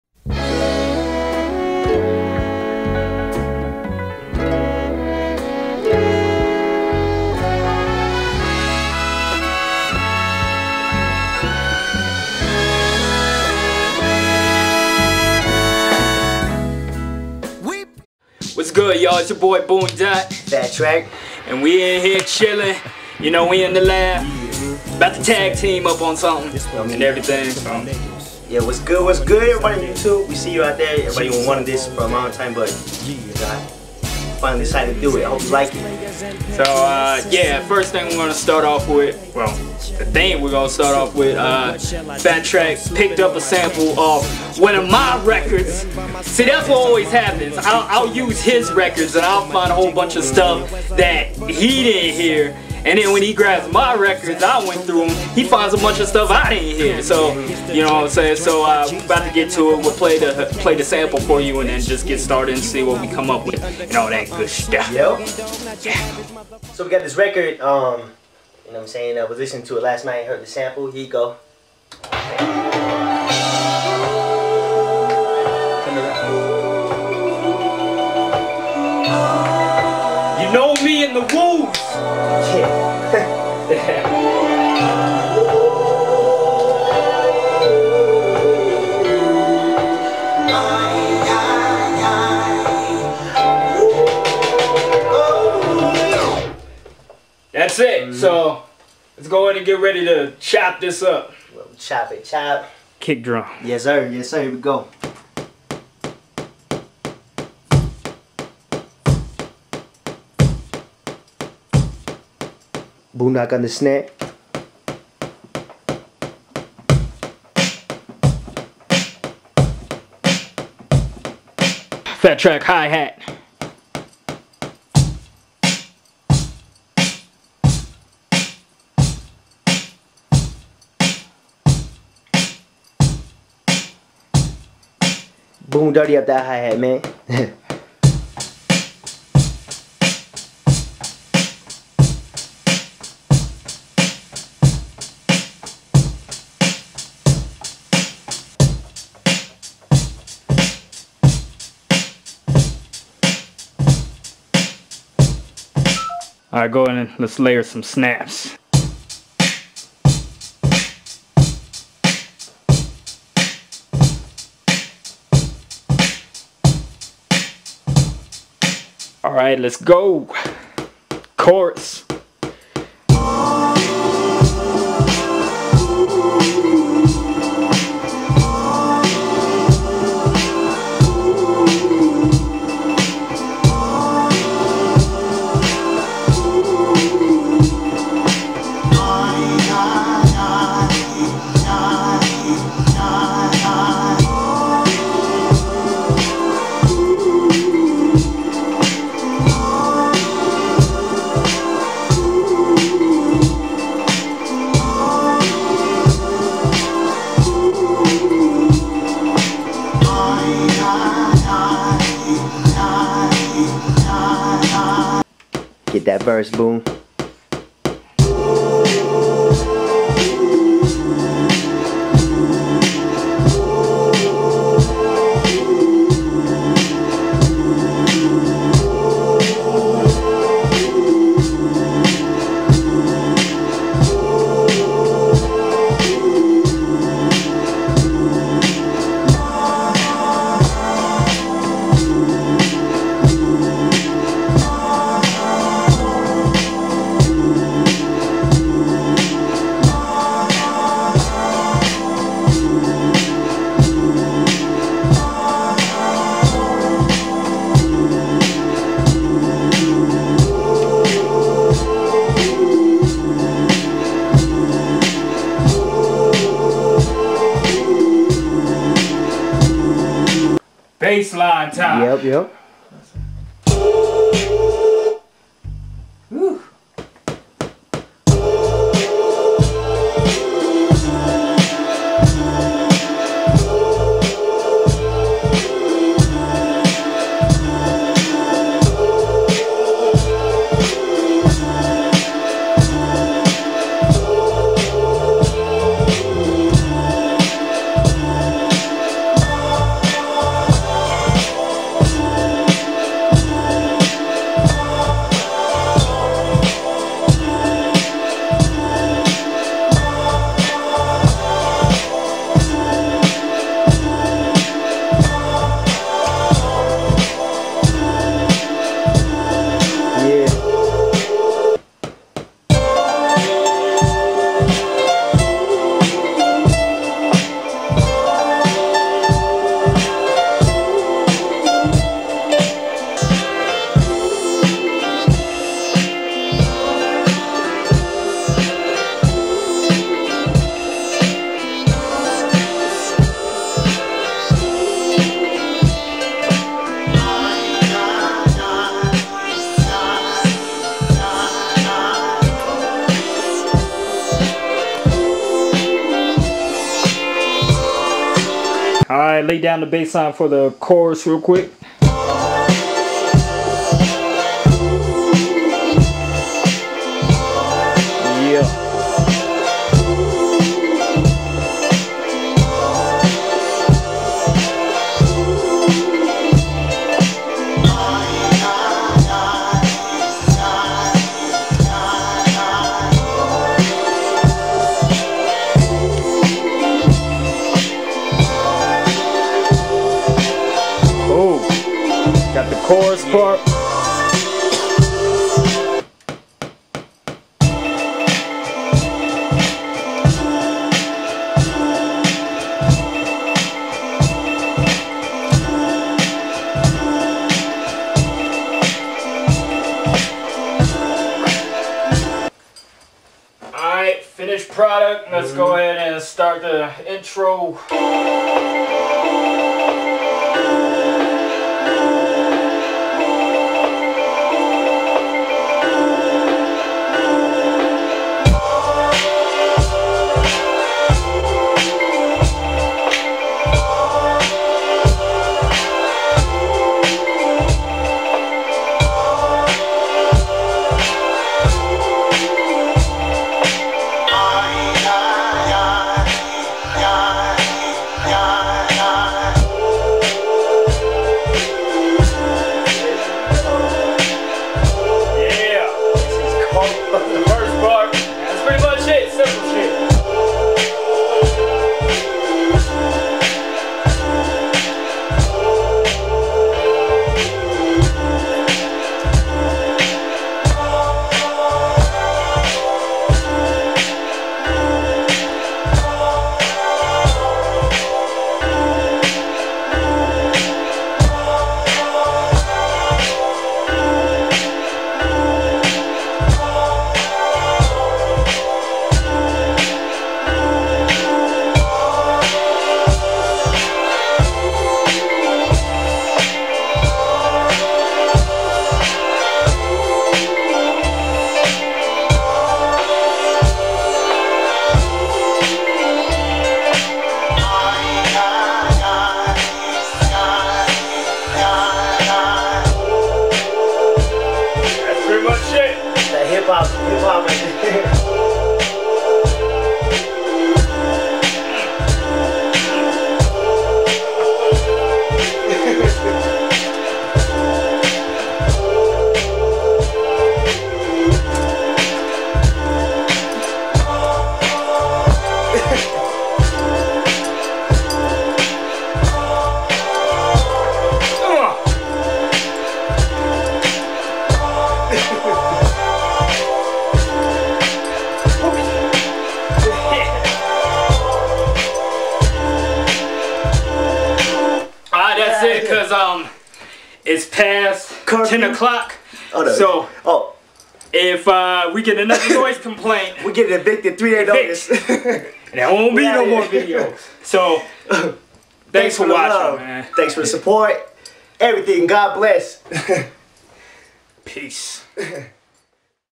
FaT TrAk and Boon Doc (TAG TEAM '08)
FaT TrAk and Boon Doc team up on a fresh new sampled beat.  Watch as they come up with heat collectively on the MPC and the Motif Rack.